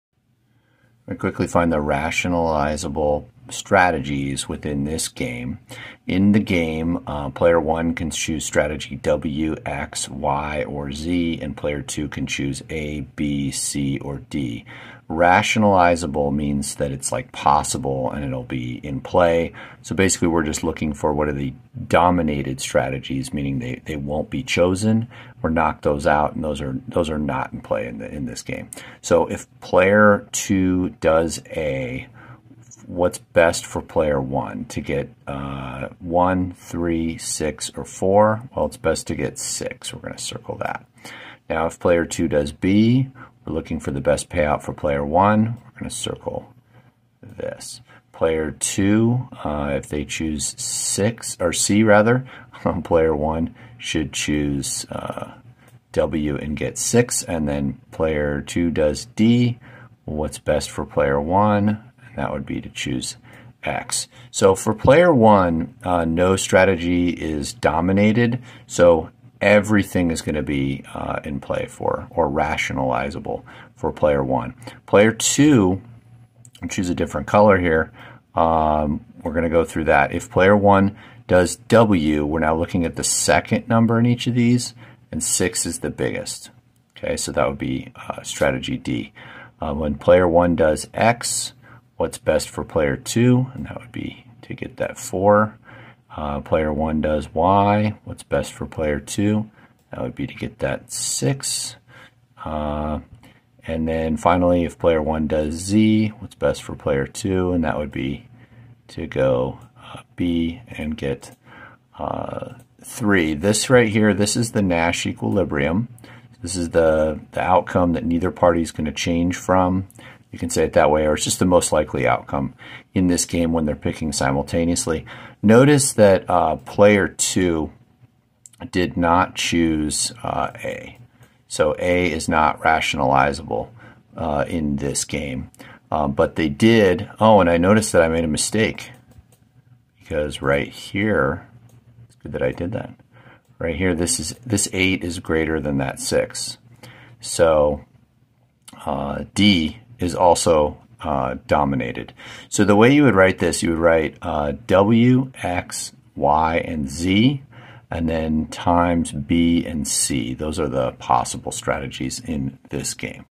How to find rationalizable strategies in a game
Find the set of rationalizable strategy profiles in the following two-player game: